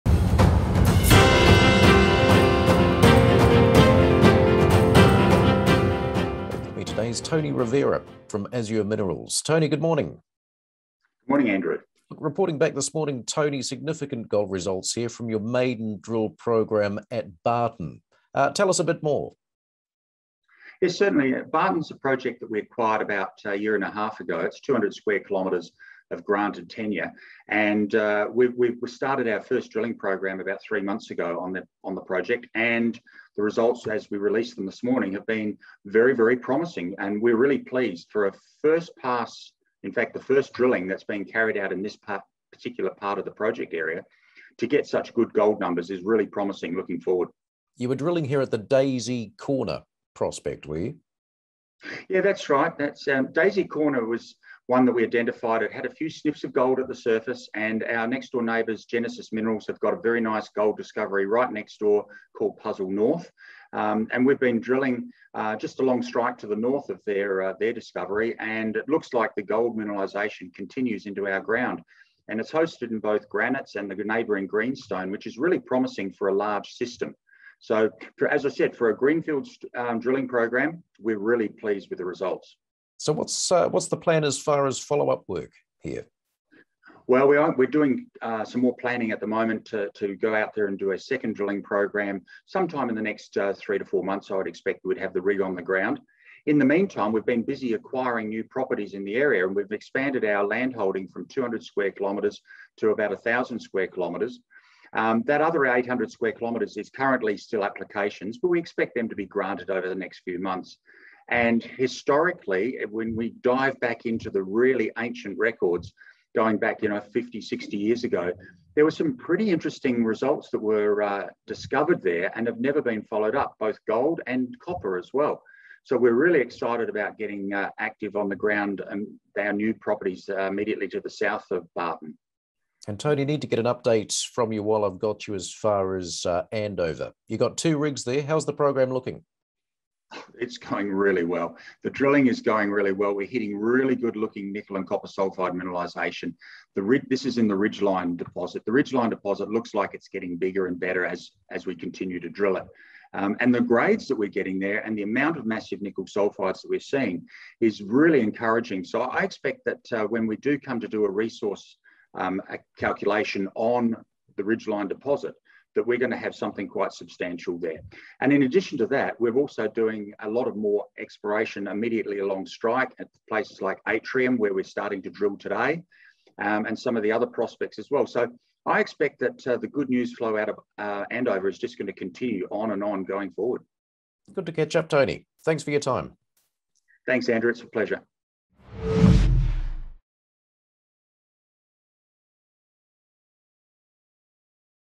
Azure Minerals intersects widespread gold mineralisation at Barton
Azure Minerals' Tony Rovira talks with Andrew Scott from Proactive about promising gold results from the company's Barton project, where it started its first drilling project three months ago. A second drilling program will start in the next three to four months. Azure has also expanded its landholding from 200 to 1000 square kilometres, with land applications pending on the additional 800 square kilometres.